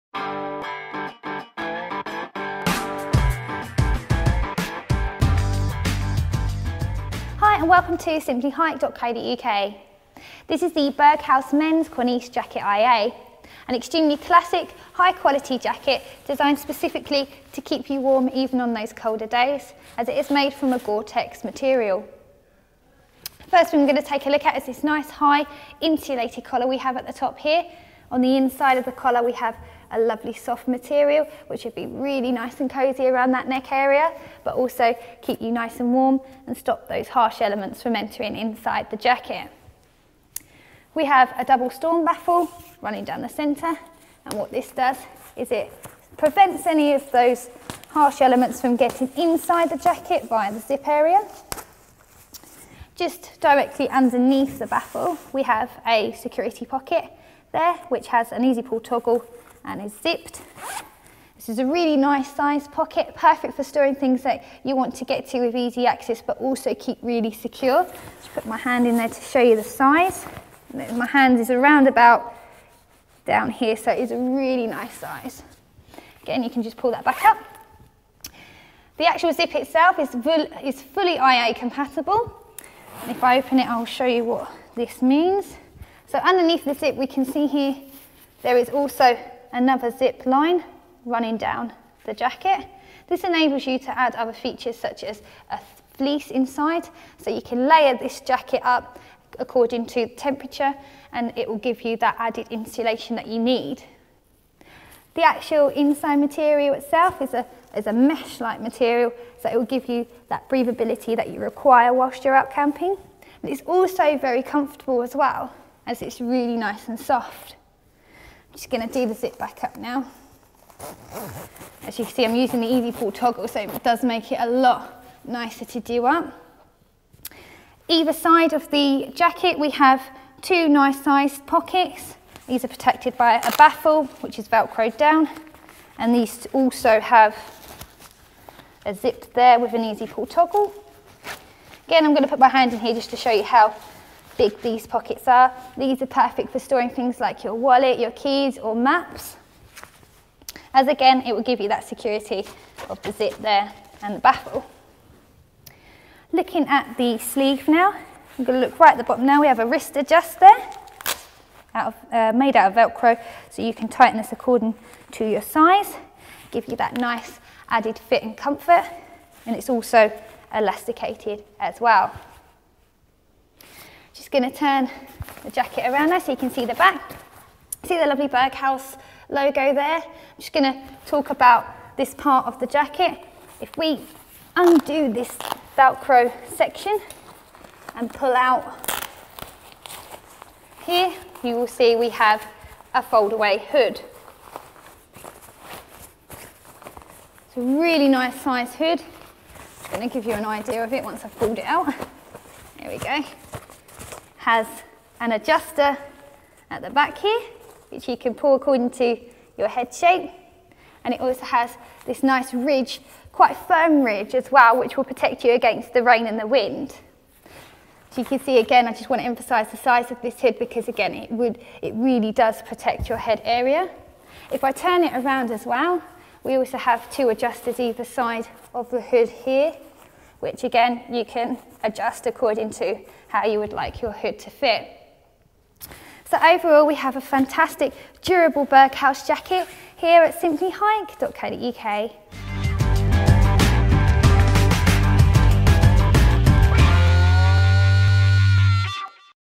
Berghaus Mens Cornice Jacket IA Eclipse
The Berghaus Mens Cornice Jacket IA Eclipse is a great, versatile jacket which encompasses Berghaus' IA system which allows other IA compatible items to be connected to the outer shell to create the best layering system for your needs. The jacket is made from a double layered Gore-Tex Performance Shell and has a mesh inner lining to ensure you are kept dry from the rain whilst retaining great breathability for when you are walking or hiking.